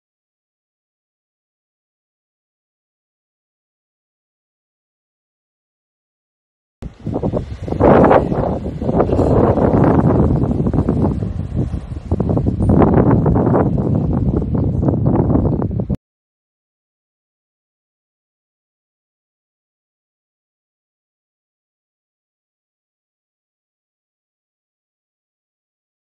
Brittle Star
Brittle Star at Flinders, Victoria on 3 January 2023.
Brittle Stars are part of the benthic (bottom feeding) fauna found in the marine environment. They are broadly referred to as Echinoderms. There are at least 40 species of brittle stars in Victorian waters. More about threatened Brittle Stars in Victoria